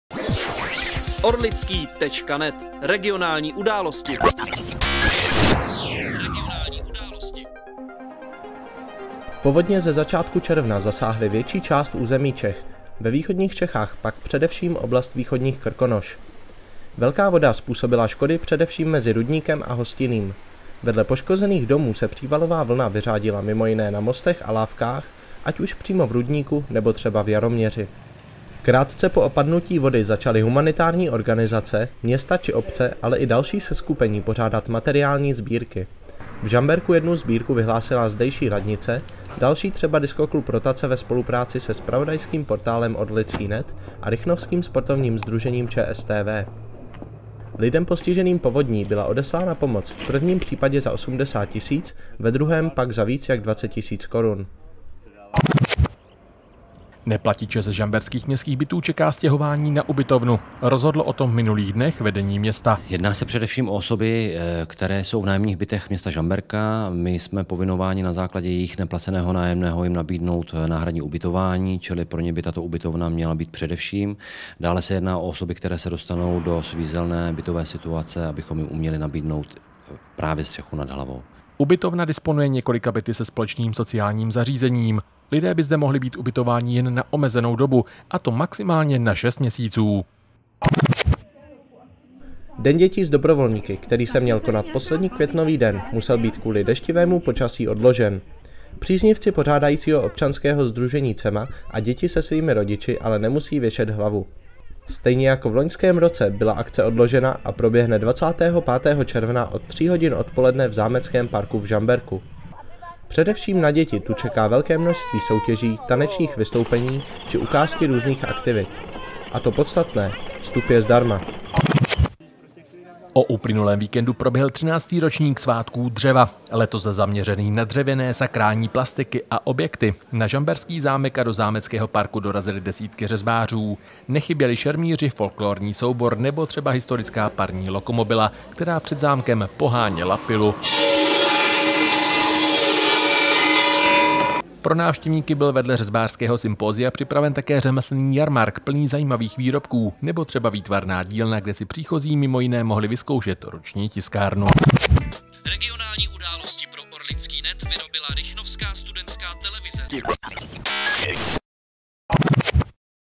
Orlický.net: Žamberské události 26-2013
Proběhla sbírka materiální pomoci pro postižené povodní • Neplatiče čeká stěhování na ubytovnu • Den dětí s dobrovolníky musel být odložen • Proběhl další ročník Svátků dřeva